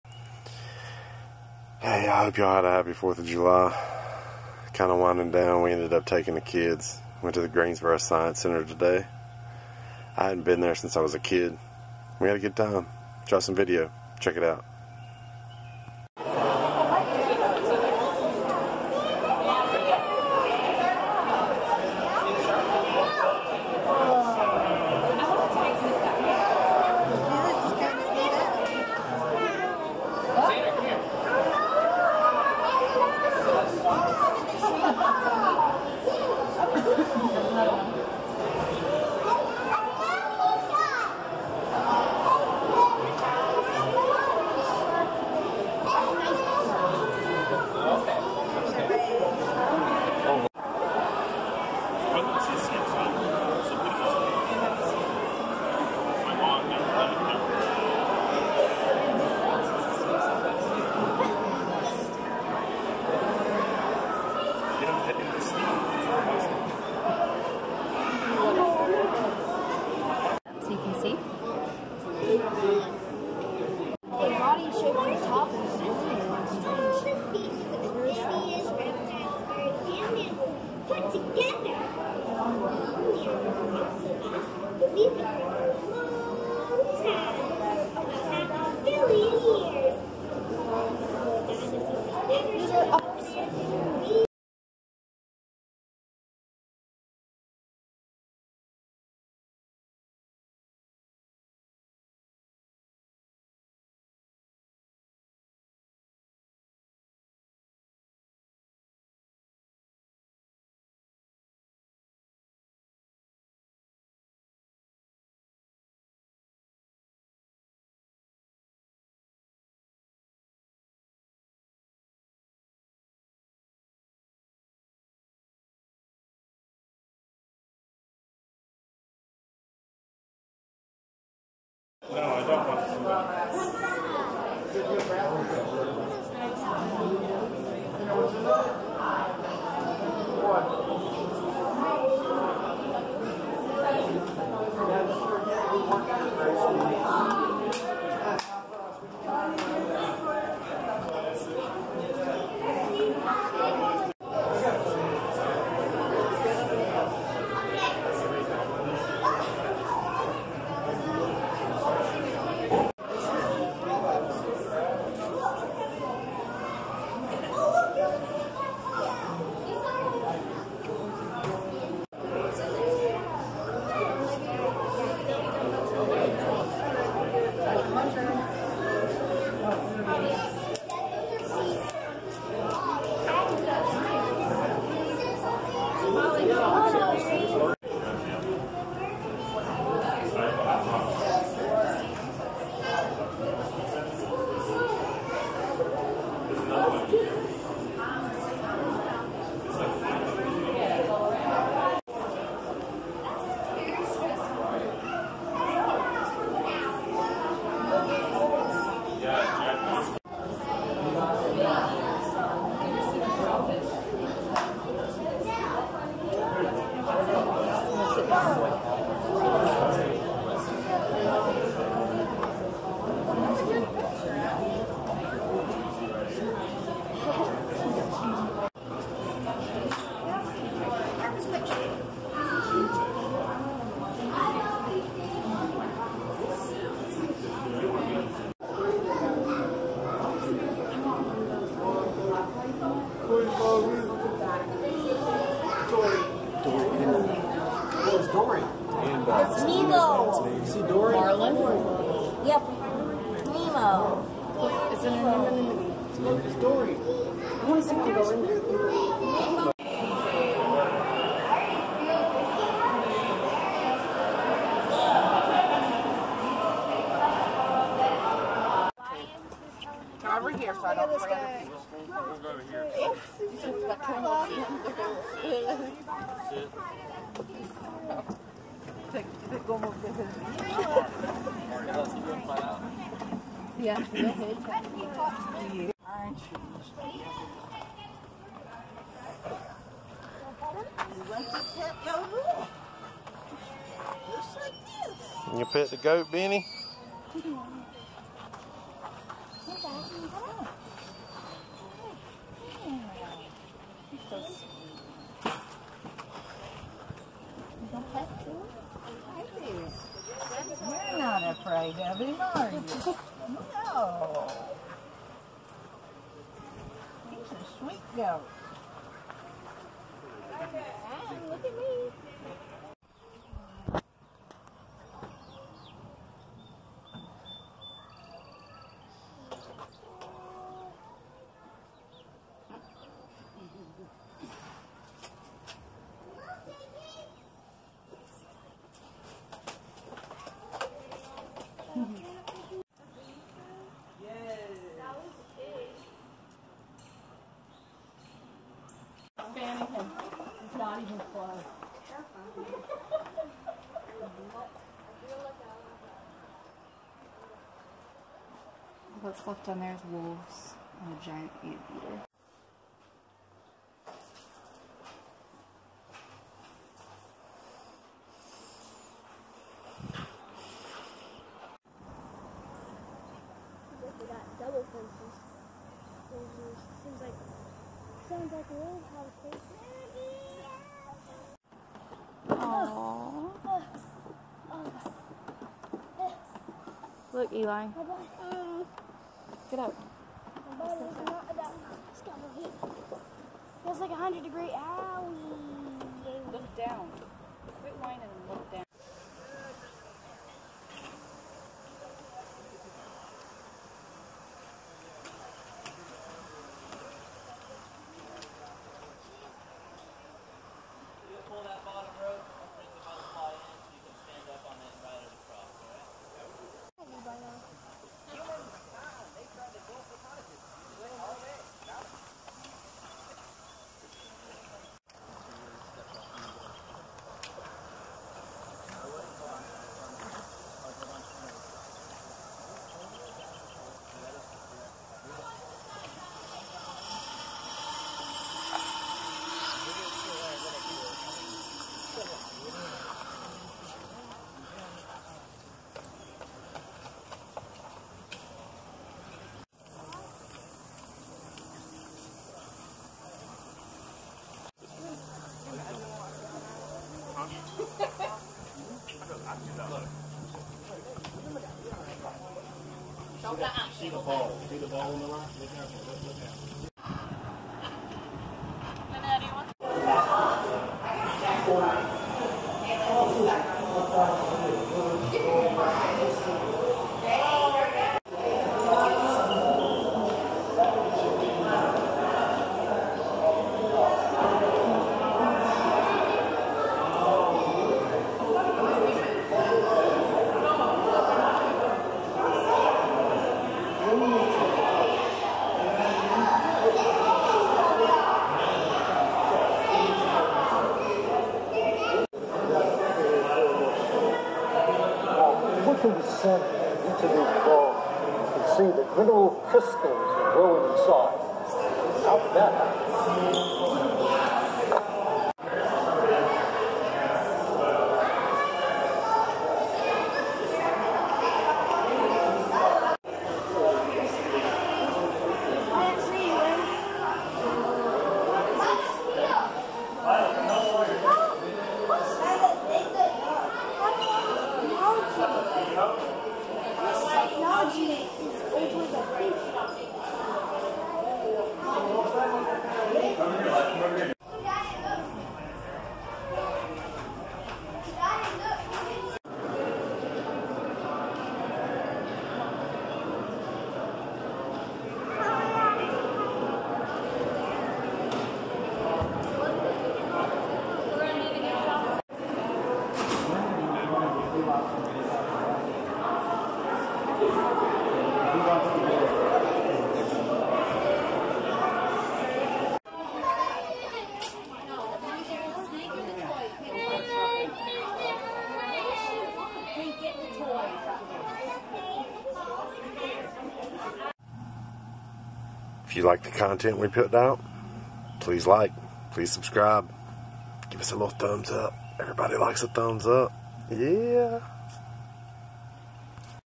Greensboro Science Center in Greensboro, NC
Culler Outdoors took a day trip to the Greensboro Science Center, Zoo, Aquarium, and Museum.  They also have a treetop adventure park which we did not do but did get to watch others do some of the course.  Great Afternoon fun for kids of all ages!